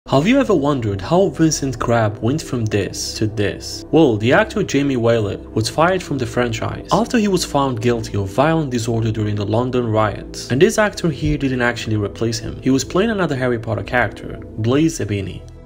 Have you ever wondered how Vincent Crabbe went from this to this in Harry Potter?
Have you ever wondered how Vincent Crabbe went from this to this? Well, the actor Jamie Waylett was fired from the franchise after he was found guilty of violent disorder during the London riots. And this actor here didn't actually replace him, he was playing another Harry Potter character, Blaise Zabini.